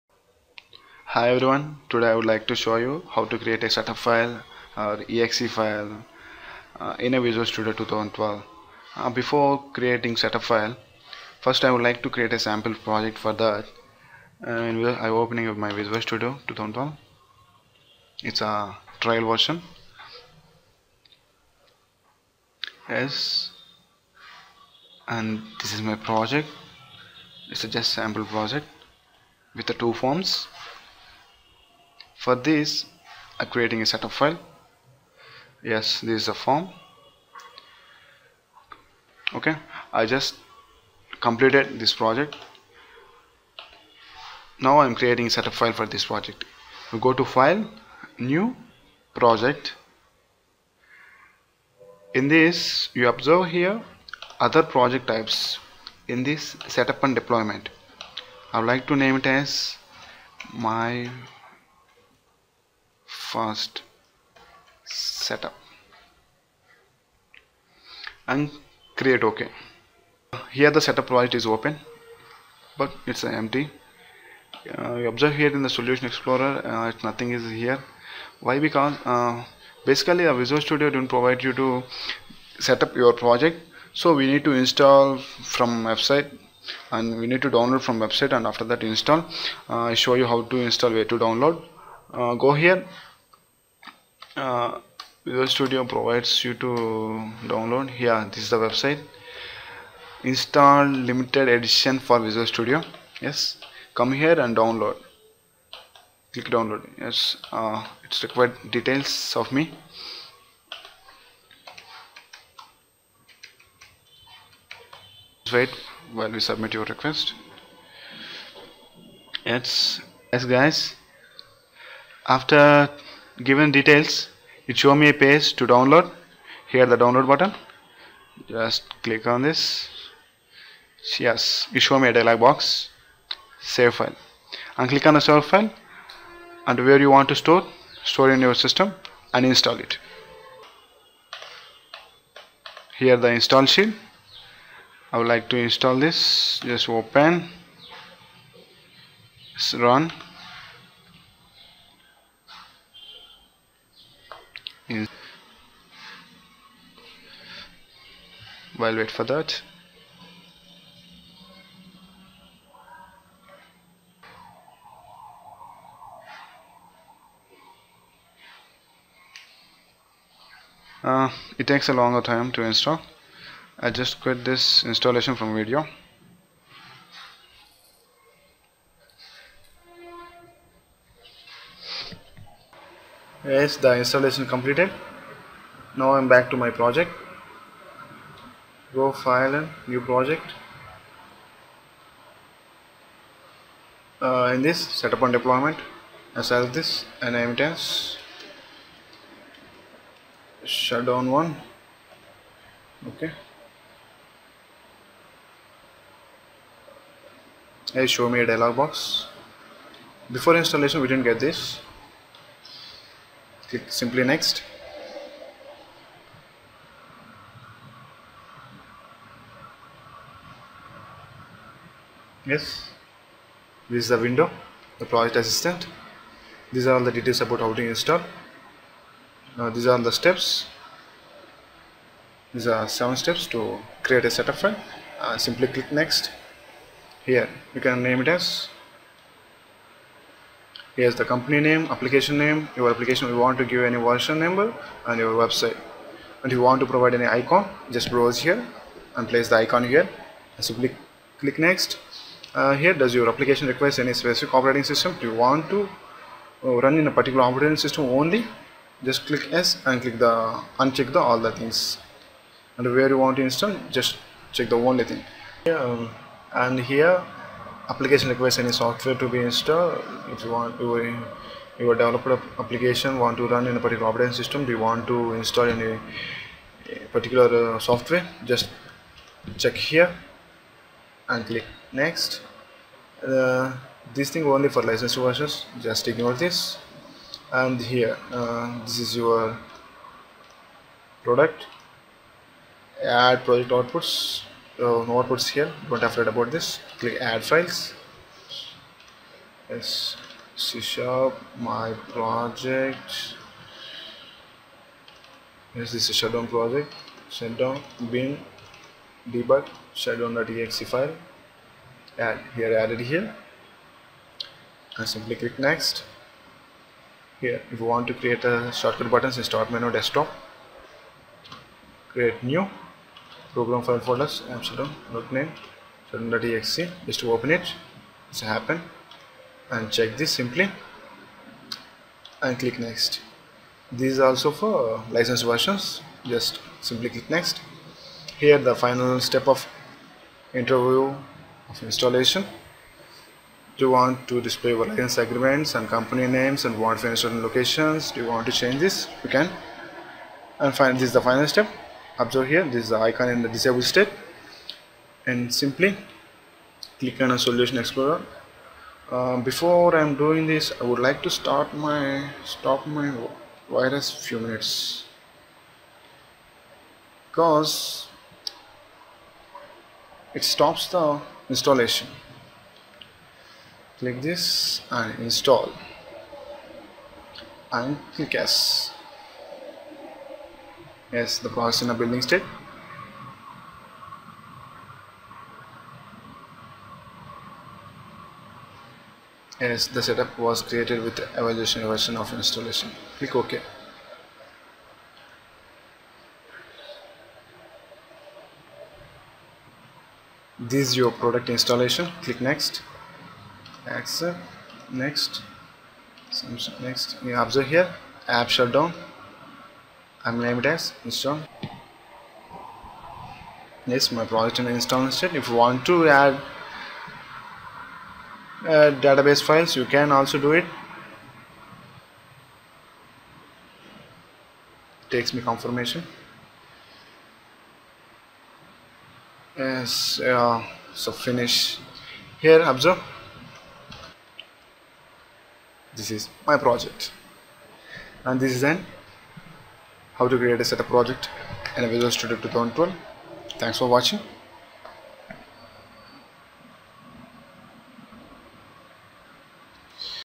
how to create setup installation form in visual studio
software exe file for install,,,
make your own software in visual studio